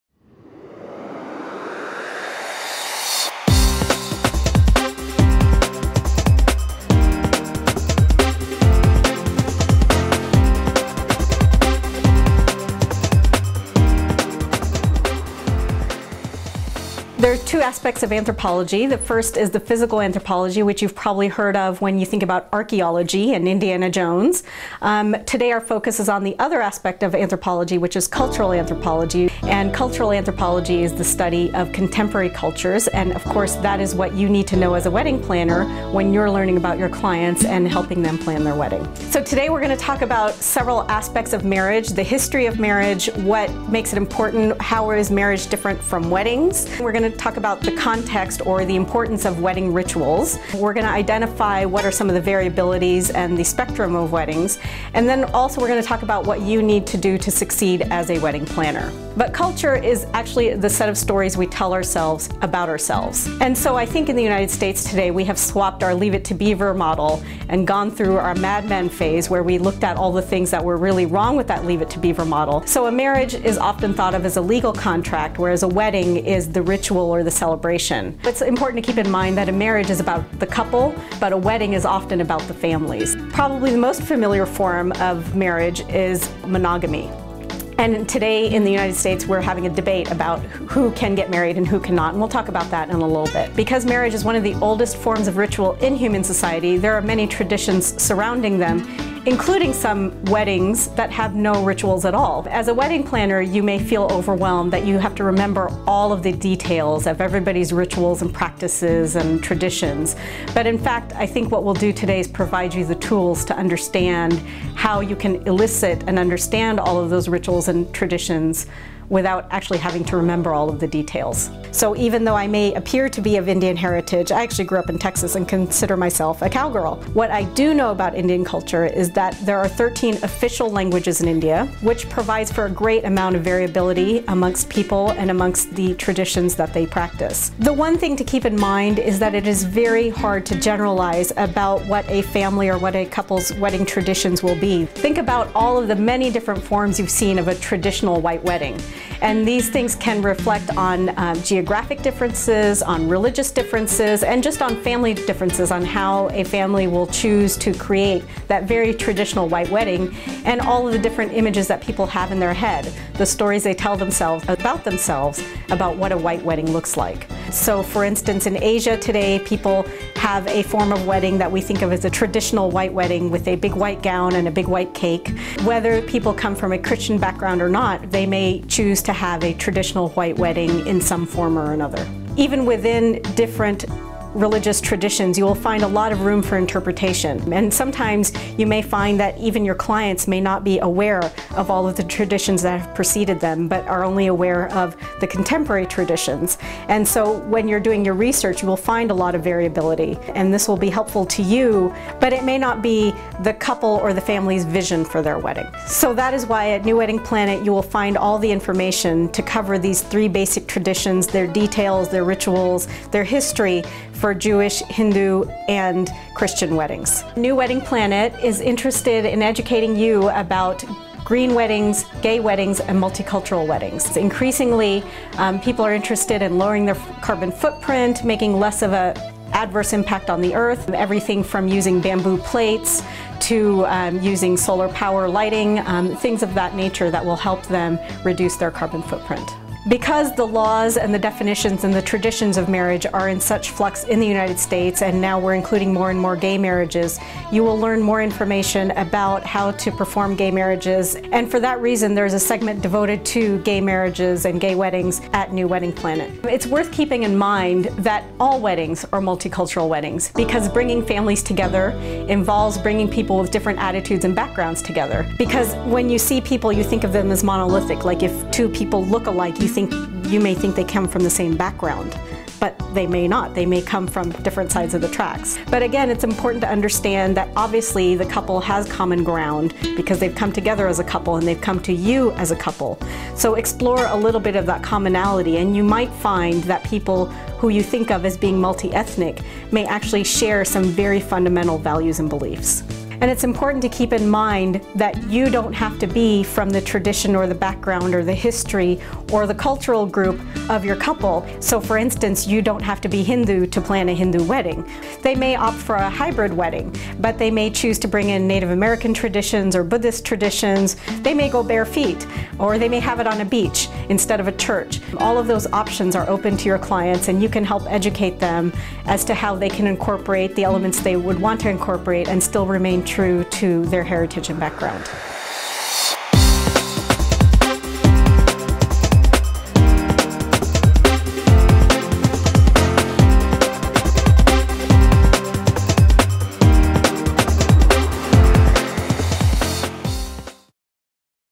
How to Become a Wedding Planner Coordinator - New Wedding Planet
Excerpts from New Wedding Planet's Course Tutorial: 'The World of Weddings'

• Learn how to become a wedding planner coordinator today

• Succeed as a wedding planner consultant for world weddings anywhere

• Find out how to be a wedding consultant from a wedding magazine publisher

• Start your career as a new wedding planner consultant with global vision

• Diversify your business by becoming a wedding planner coordinator for world weddings in your community

• Plan your wedding with insider tips on how to be a wedding consultant for your own wedding, and for your friends'

• An introduction to world weddings by an anthropologist and New Wedding Planet wedding trends expert